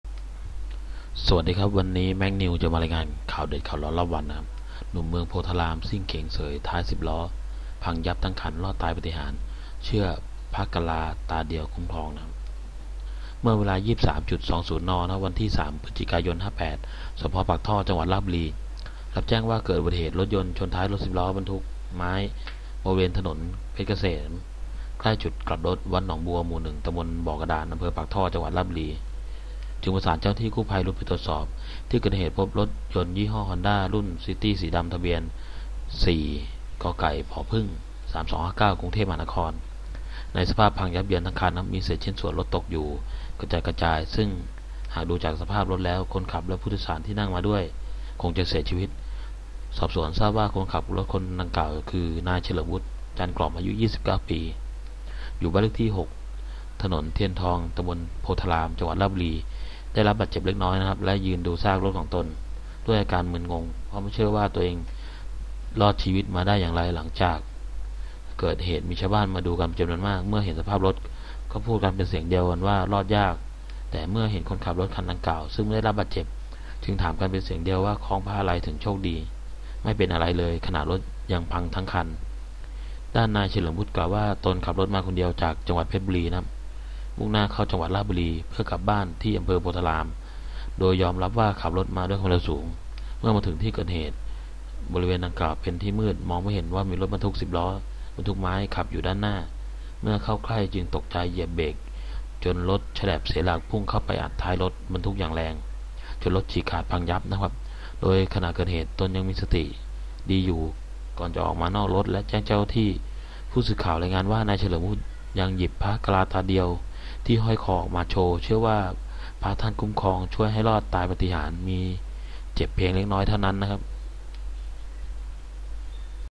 หนุ่มเมืองโพธารามซิ่งเก๋งเสยท้าย 10 ล้อพังยับทั้งคันรอดตายปาฏิหาริย์ เชื่อพระกะลาตาเดียวคุ้มครอง
เมื่อเวลา 23.20 น. วันที่ 3 พ.ย. สภ.ปากท่อ จ.ราชบุรี รับแจ้งว่าเกิดอุบัติเหตุรถยนต์ชนท้ายรถสิบล้อบรรทุกไม้ บริเวณถนนพชรเกษม ใกล้จุดกลับรถ วัดหนองบัว หมู่ 1 ต.บ่อกระดาน อ.ปากท่อ จ.ราชบุรี จึงประสานเจ้าหน้าที่กู้ภัยรุดไปตรวจสอบ

 ที่เกิดเหตุพบรถยนต์ยี่ห้อ ฮอนด้า รุ่นซิตี้ สีดำ ทะเบียน 4กผ 3259 กรุงเทพมหานคร ในสภาพพังยับเยินทั้งคัน มีเศษชิ้นส่วนรถตกอยู่กระจัดกระจาย ซึ่งหากดูจากสภาพรถแล้ว คนขับและผู้โดยสารที่นั่งมาด้วยคงจะเสียชีวิต
 สอบสวนทราบว่า คนขับรถคันดังกล่าวคือ นายเฉลิมวุฒิ จันทร์กล่อม อายุ 29 ปี อยู่บ้านเลขที่ 6 ถ.เทียนทอง ต.โพธาราม จ.ราชบุรี ได้รับบาดเจ็บเล็กน้อย และยืนดูซากรถของตนเอง ด้วยอาการมึนงง เพราะไม่เชื่อว่าตัวเองรอดชีวิตมาได้อย่างไร หลังจากเกิดเหตุมีชาวบ้านมาดูกันเป็นจำนวนมาก เมื่อเห็นสภาพรถก็พูดกันเป็นเสียงเดียวว่ารอดยาก แต่เมื่อเห็นคนขับรถคันดังกล่าว ซึ่งไม่ได้รับบาดเจ็บ จึงถามกันเป็นเสียงเดียวว่าคล้องพระอะไร ถึงโชคดี ไม่เป็นอะไรเลย ขนาดรถยังพังทั้งคัน

 ด้านนายเฉลิมวุฒิ กล่าวว่า ตนขับรถมาคนเดียวจากจ.เพรชบุรี ม่งหน้าเข้าจ.ราชบุรี เพื่อจะกลับบ้านที่อ.โพธาราม โดยยอมรับว่าขับมาด้วยความเร็วสูง เมื่อมาถึงที่เกิดเหตุบริเวณดังกล่าวเป็นที่มืด มองไม่เห็นว่ามีรถบรรทุก 10 ล้อบรรทุกไม้ ขับอยู่ด้านหน้าเมื่อเข้าใกล้จึงตกใจ เหยียบเบรคจนรถแฉลบเสียหลักพุ่งเข้าไปอัดท้ายรถบรรทุกอย่างแรง จนรถฉีกขาดพังยับ โดยขณะเกิดเหตุ ตนยังมีสติดีอยู่ก่อนจะออกมานอกรถและแจ้งเจ้าหน้าที่

 ผู้สื่อข่าวรายงานว่า นายเฉลิมวุฒิ ยังหยิบพระกะลาตาเดียวที่ห้อยคอออกมาโชว์ เชื่อว่าพระท่านคุ้มครองช่วยให้รอดตายปาฏิหาริย์ มีเจ็บเพียงเล็กน้อยเท่านั้น